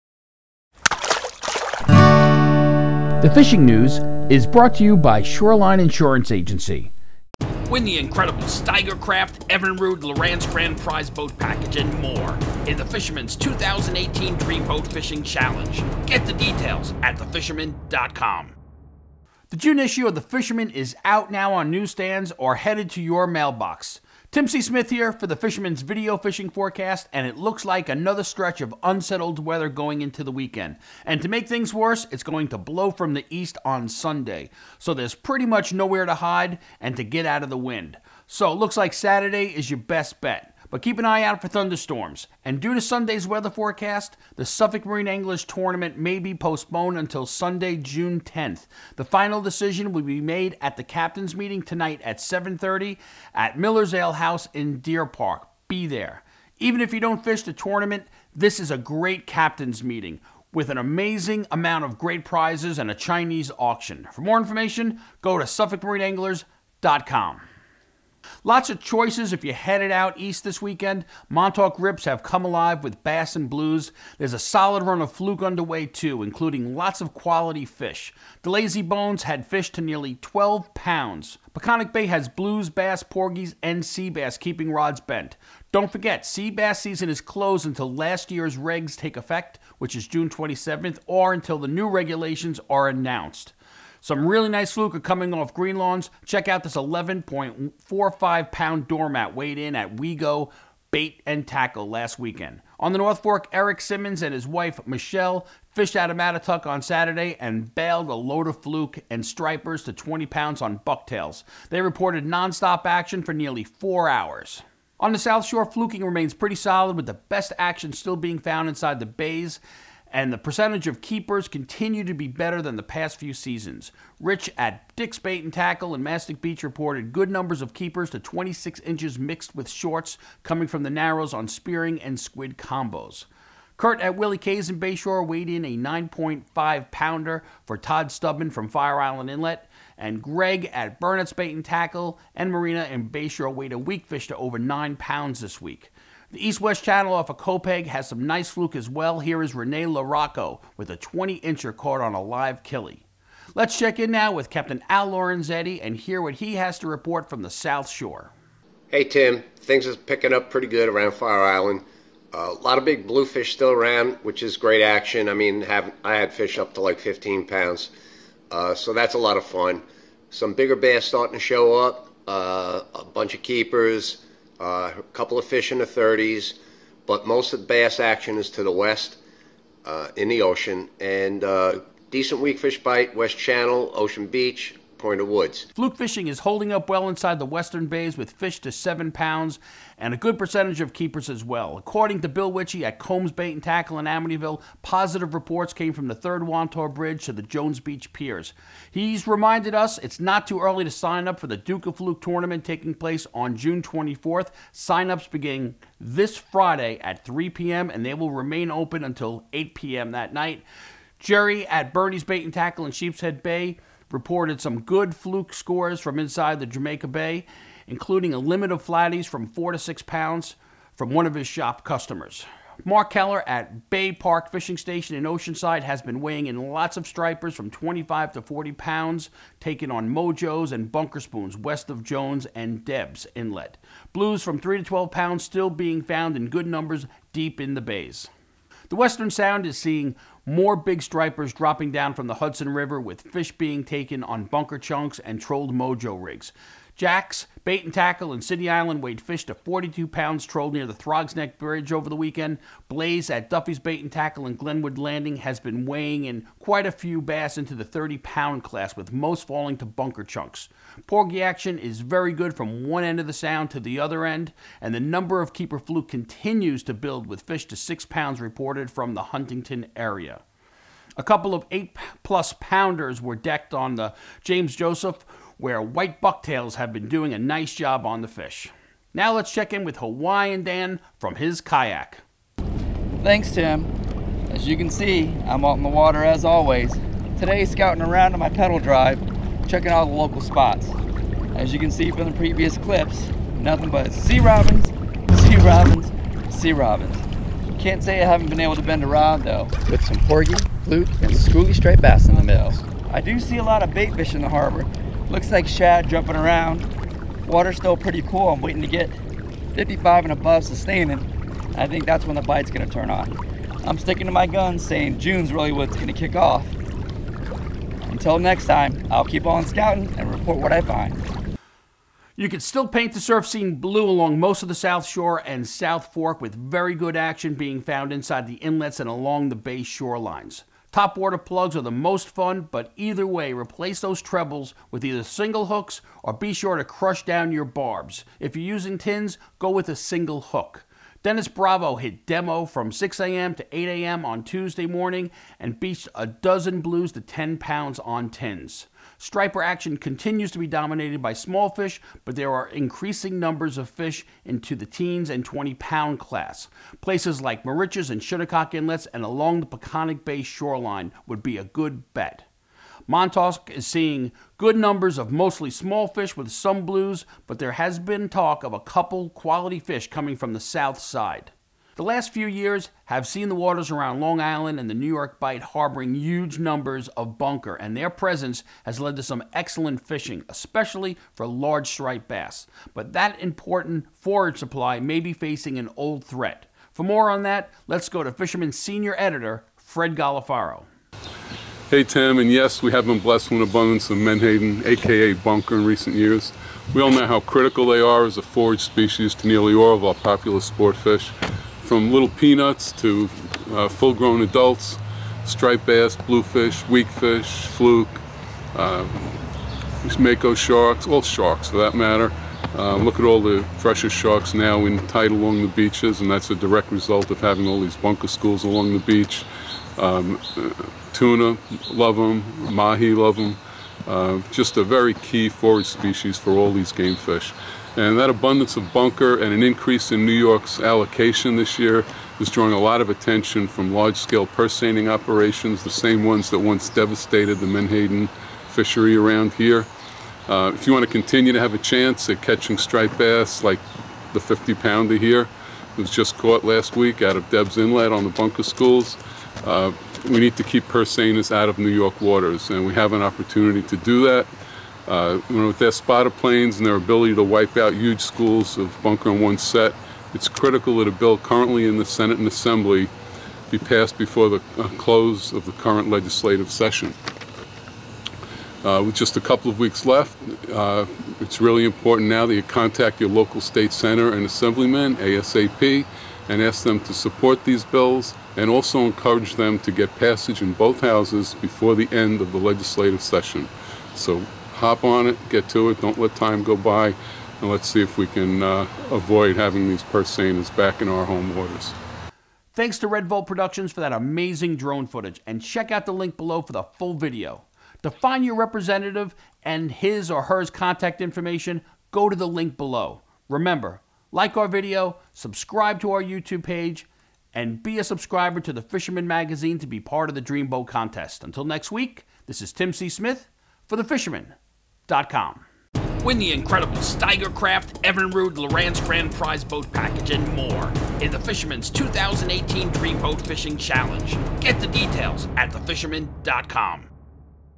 May 31, 2018 Long Island Metro Fishing Report with Fred Golofaro
Another weekend with lots of choices when it comes to where you want to put your effort, and what you want to target. East End options include stripers in the Montauk rips and surf, a quickly developing fluke run in Montauk and the Peconics, weakfish in Noyack Bay, and cod if you’re looking to make up for the winter’s lackluster run.

From Shinnecock to Sheepshead Bay, you can target the bays for fluke where the keeper ration has been better than what we’ve come to expect the last few years. The bigger concentration of cow stripers seems to be holding west of Jones, but the action has been spreading east, and there is no shortage of smaller bass for those who want to ladle clam chum. Weakfish are a solid option here as well, with the Point O’Woods to Ocean Beach area in Great South Bay certainly worth an early morning shot.

In the Sound, the fluke fishing is definitely worthy of some attention, especially in the mid-island area, where there seem to be some better quality fish. Stripers have spread well to the east, but here too, the bigger fish seem concentrated to the west, from Huntington to the Throgs Neck Bridge.

Now, if you are wondering about porgies and bluefish? Porgies are spread from one end of the Sound to the other. They remain thick in Peconic Bay, Gardiners and Cherry Harbor. Bluefish on the other hand, remain well entrenched all through the South Shore bays and inlets and have made their way to Montauk. Big numbers have yet to reach the Sound but could change by the time you read this.

And then there are sea bass. Yes the season is closed but in many areas they are impossible to avoid with many anglers getting sick over having to throwback numerous 3 to 5-plus pounders. We’re still waiting to see if there will be any changes to last year’s regs in which the season opened on June 27th.